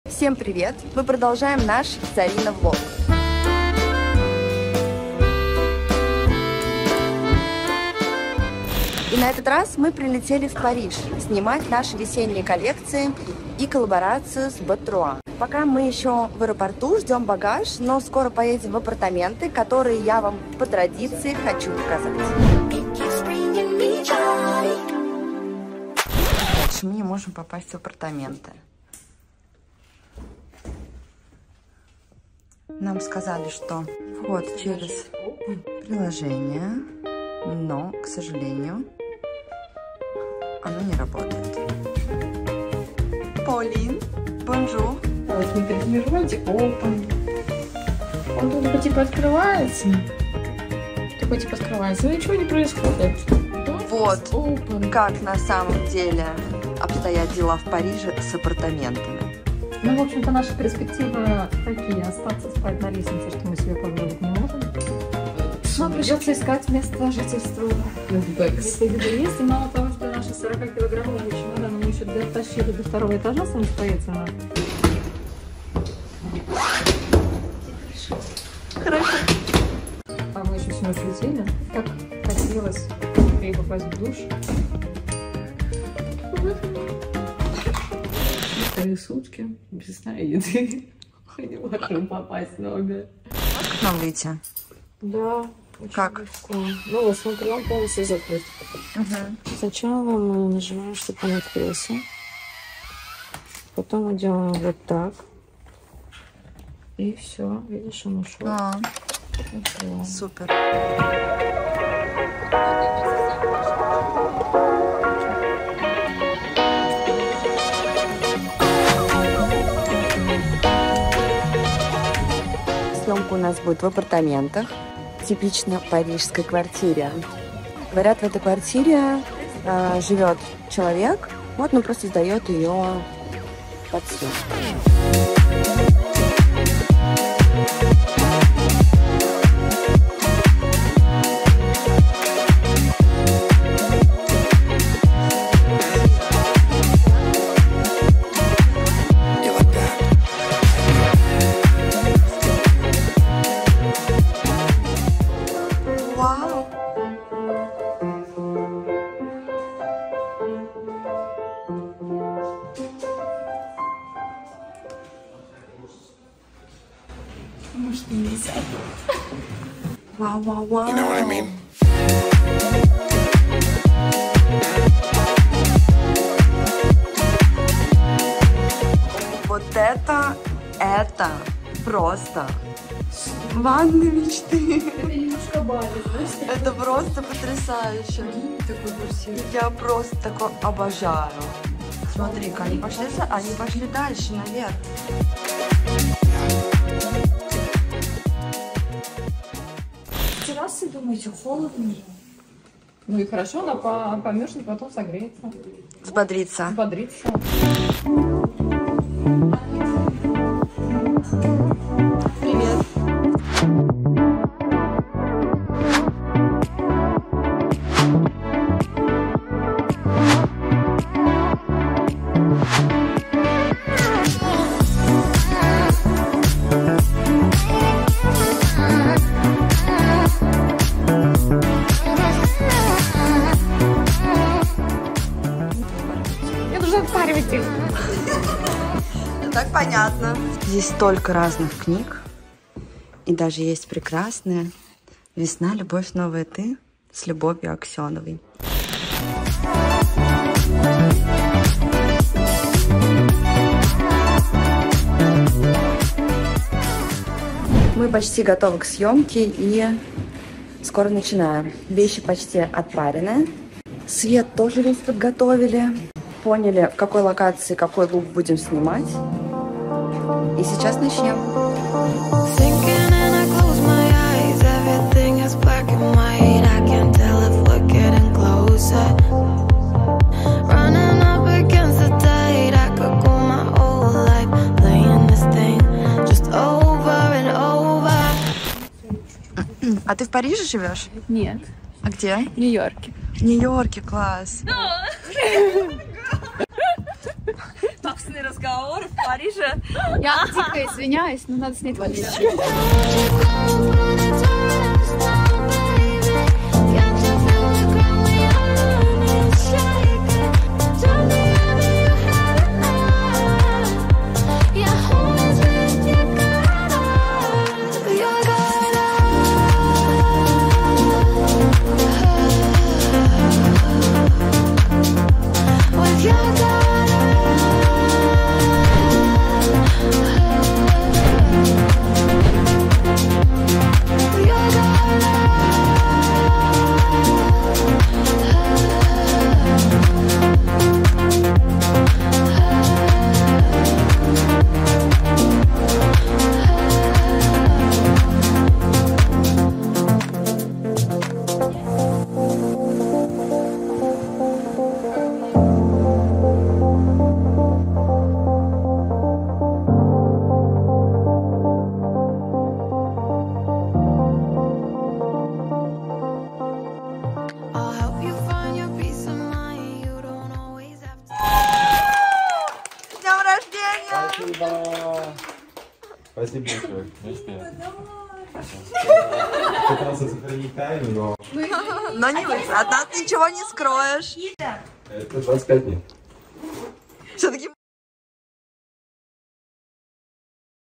ZARINA VLOG / Paris
Влог ZARINA из Парижа 🤍
Съёмка весенних новинок останется не только на стильных фото но и в наших сердцах: яркие образы, живописные локации и романтичная атмосфера французских апартаментов. Смотри наш влог и вдохновляйся творческим съёмочным процессом!